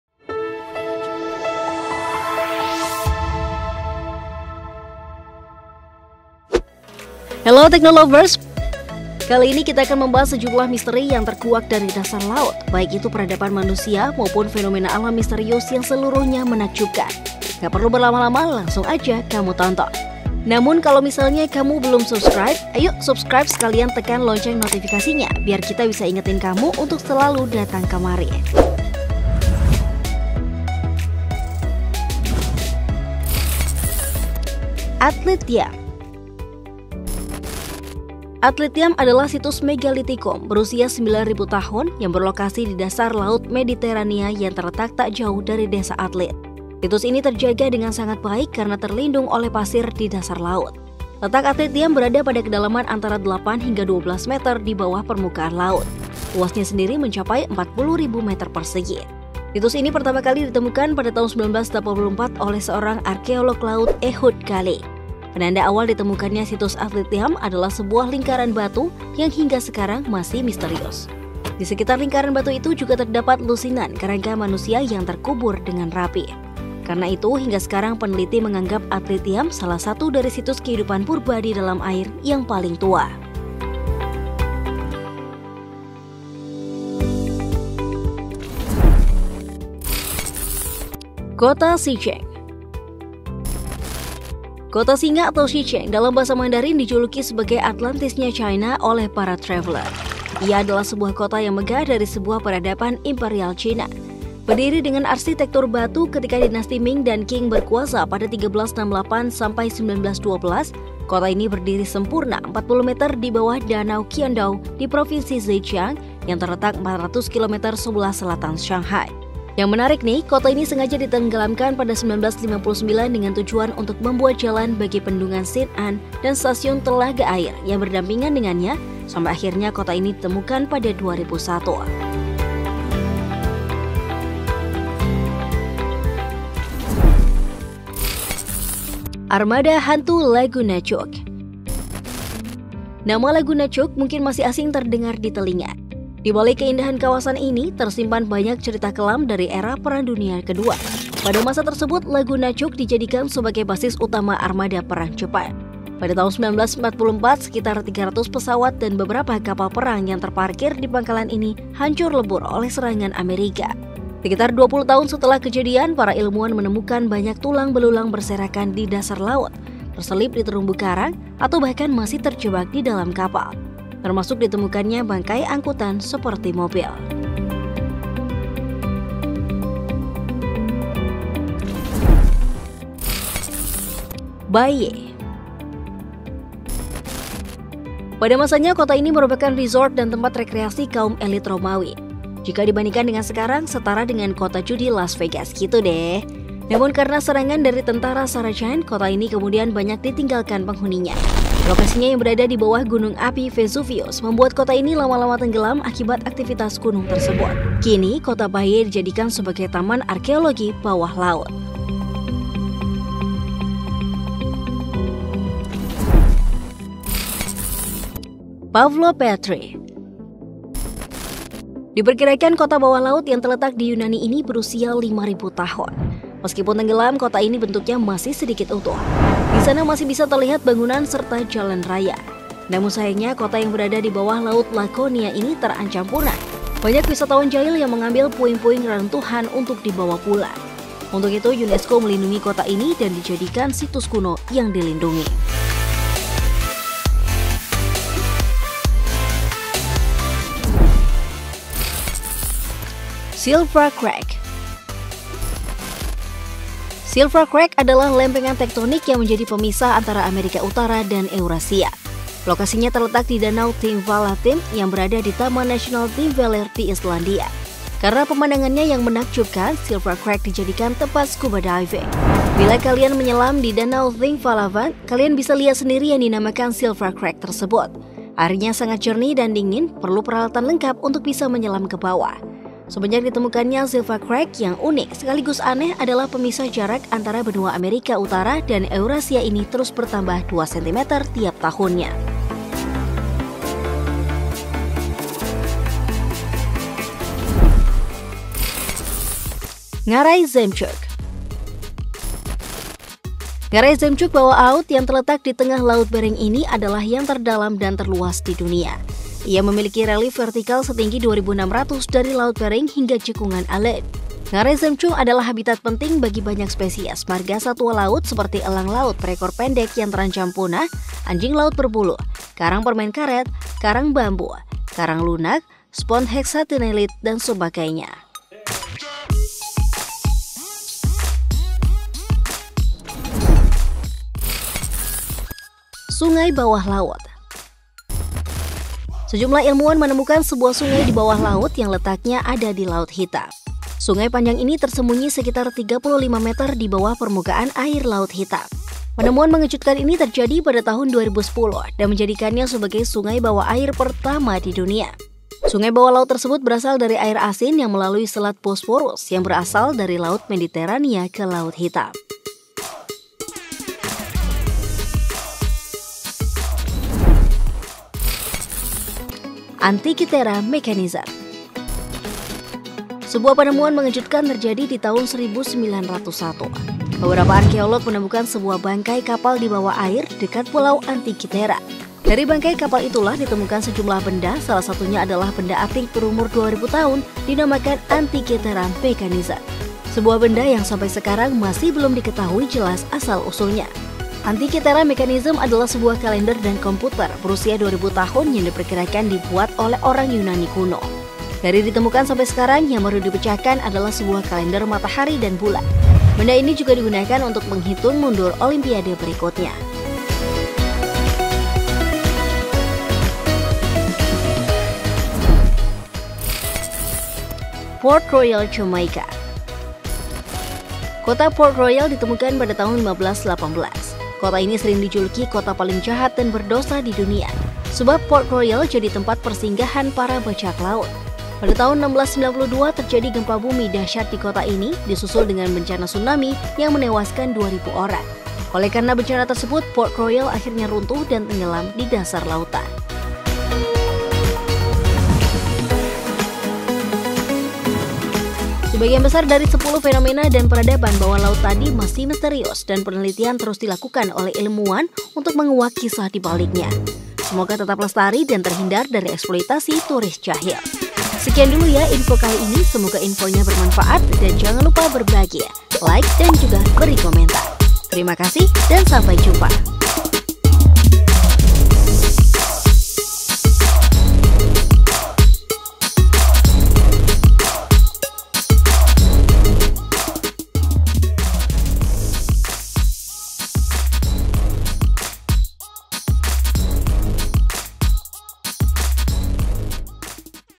Penemuan Lubang Misterius Bawah Laut, Setelah DiTeliti Tim Penyelam Temukan Hal Mengejutkan
Apakabar hari ini? Kali ini kita akan bahas sejumlah misteri yang dikuak dari dasar laut, baik itu
peradaban manusia maupun fenomena alam misterius yang seluruhya menakjubkan. Gak perlu
berlama-lama langsung aja kamu tonton.